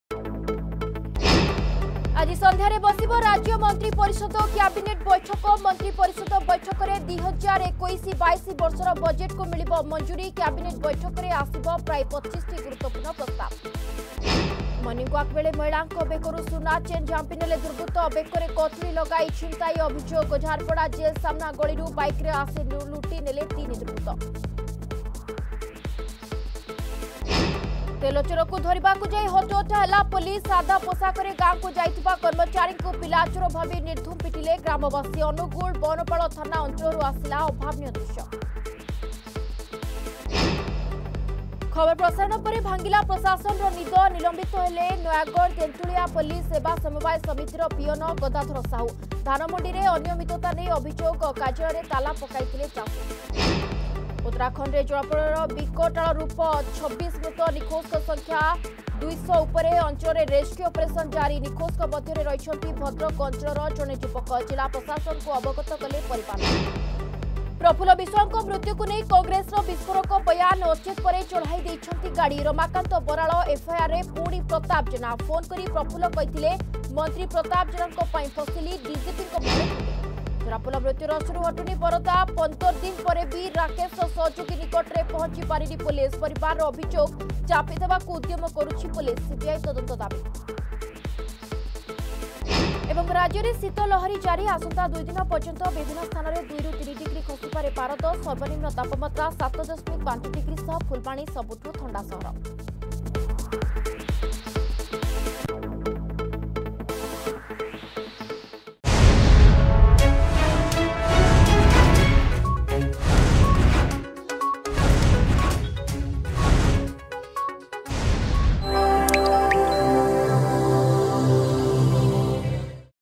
Headlines 9Am
ଆଜି ସଂଧ୍ୟାରେ ବସିବ ରାଜ୍ୟ ମନ୍ତ୍ରୀ ପରିଷଦ ଓ କ୍ୟାବିନେଟ ବୈଠକ…
ମର୍ଣ୍ଣିଂୱାକ ବେଳେ ମହିଳାଙ୍କ ବେକରୁ ସୁନା ଚେନ ଝାମ୍ପି ନେଲେ ଦୁର୍ବୃତ୍ତ...
ତେଲ ଚୋରକୁ ଧରିବାକୁ ଯାଇ ହଟହଟା ହେଲା  ପୋଲିସ...
ଖବର ପ୍ରସାରଣ ପରେ ଭାଙ୍ଗିଲା ପ୍ରଶାସନର ନିଦ...
ଉତ୍ତରାଖଣ୍ଡରେ ଜଳପ୍ରଳୟର ବିକଟାଳ ରୂପ.
ପ୍ରଫୁଲ୍ଲ ବିଶ୍ବାଳଙ୍କ ମୃତ୍ୟୁକୁ ନେଇ କଂଗ୍ରେସର ବିସ୍ଫୋରକ ବୟାନ.
ଝରାଫୁଲ ମୃତ୍ୟୁ ରହସ୍ୟରୁ ହଟୁନି ପର୍ଦ୍ଦା...
ରାଜ୍ୟରେ ଶୀତ ଲହରୀ ଜାରି...